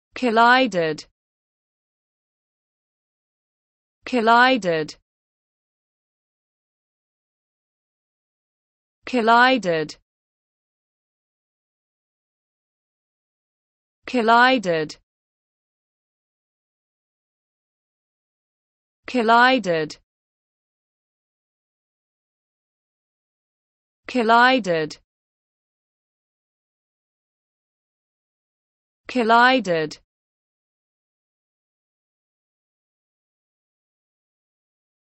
the correct pronunciation of collided in English.
This video shows you how to pronunciation collided in English.